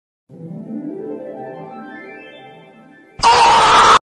Bumblebee screams at you (real)
Bumblebee reacts to literally existing. Funny Wings of Fire meme I made in Flipaclip.